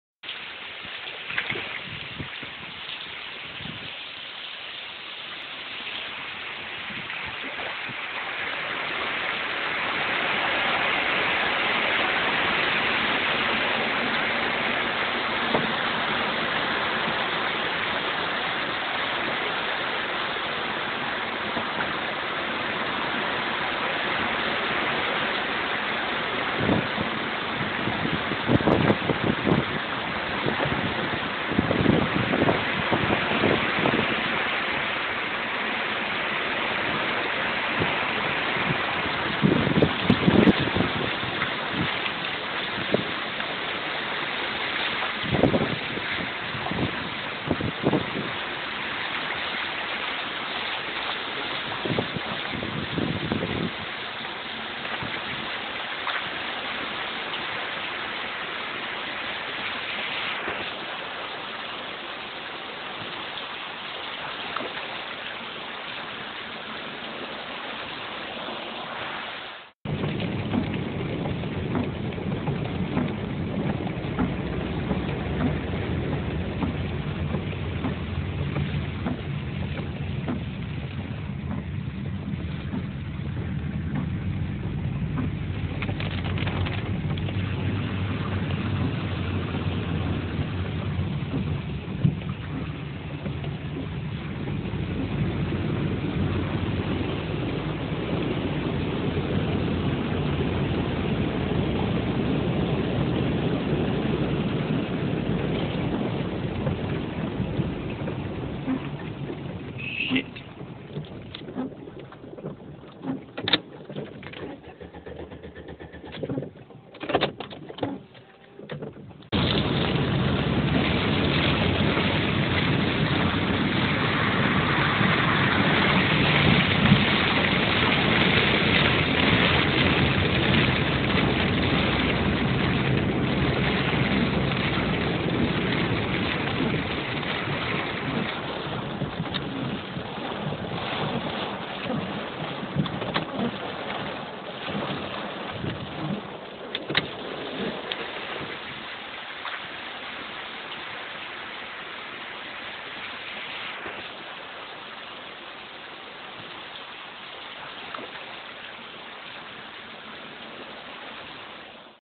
How to destroy your car's engine.
This was back in 2012 during Hurricane Sandy. I'm older and wiser now but I'm still a little embarrassed to admit doing this. I figure if someone else can learn from my mistake though then that's a good thing.